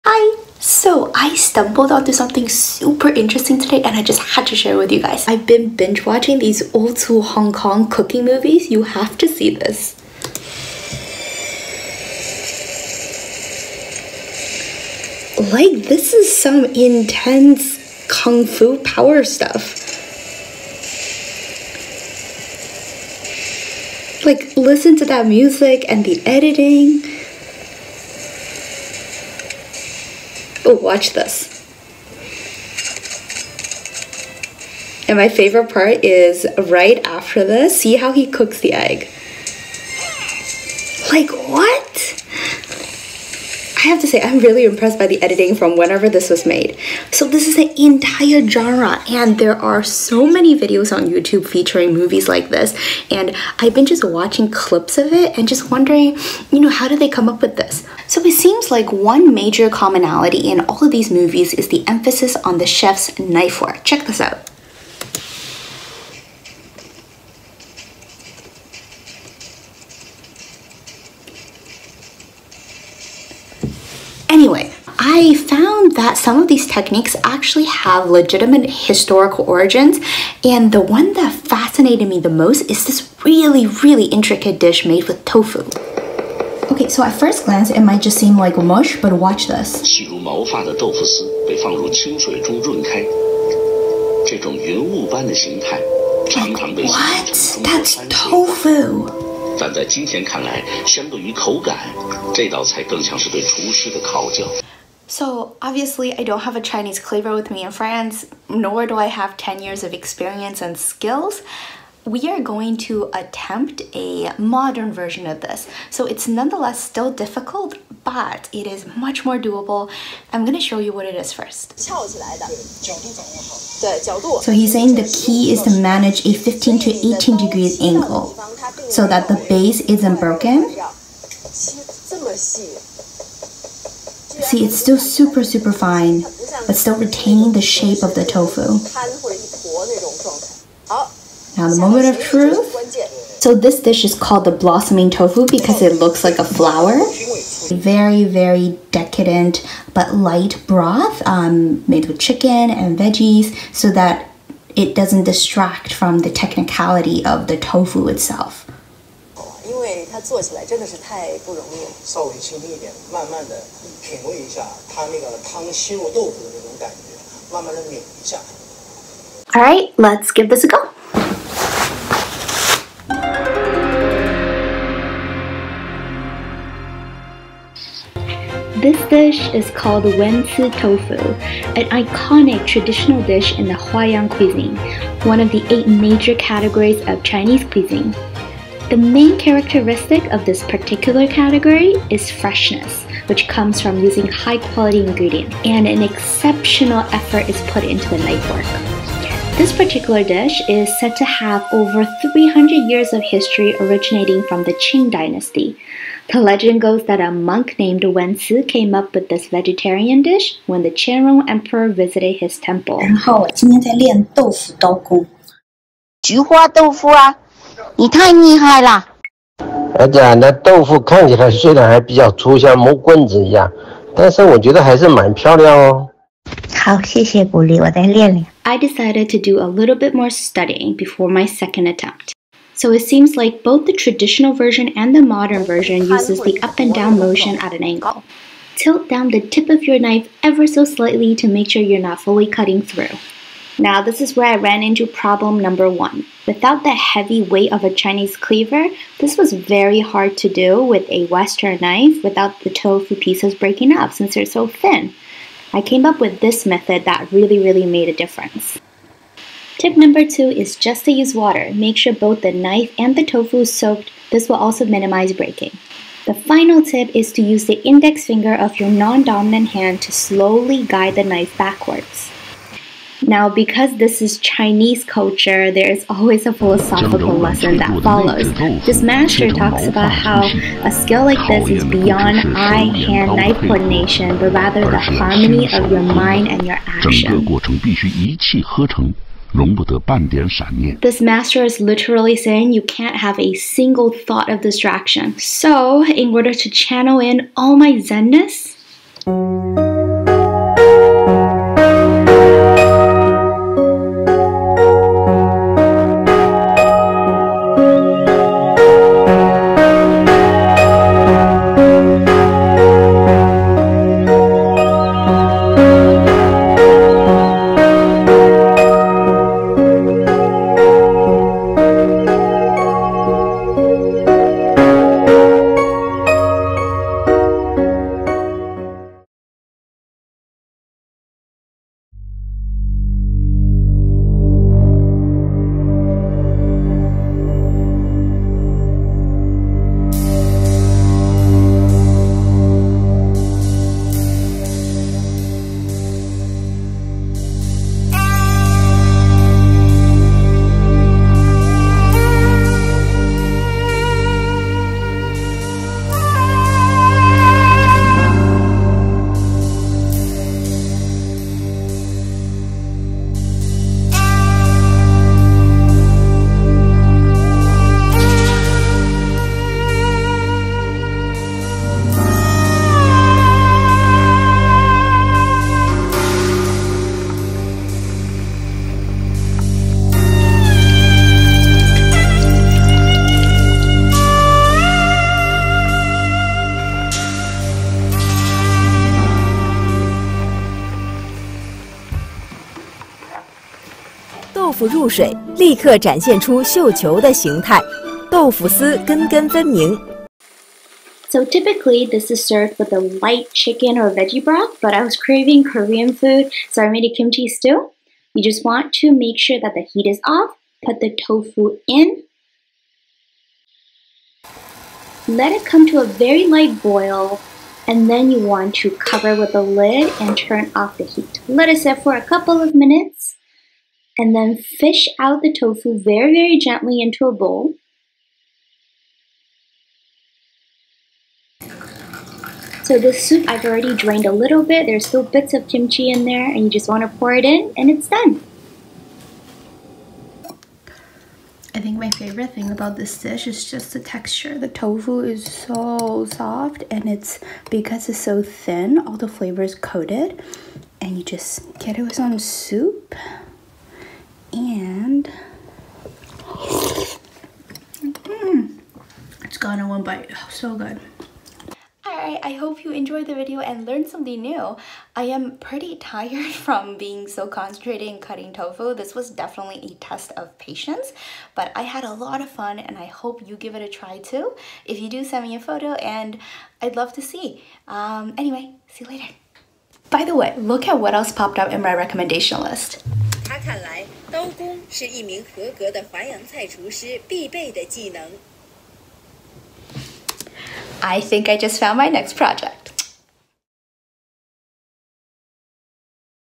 【Diary of a chef in training】Ancient Qing Dynasty Chinese Knife Skill - Blossoming Tofu
I stumbled onto some very cool knife techniques by Chinese masterchefs and decided to attempt one! Detailed tips includes on how you can do this too at home! Oh and if you try this, make sure you are using soft tofu!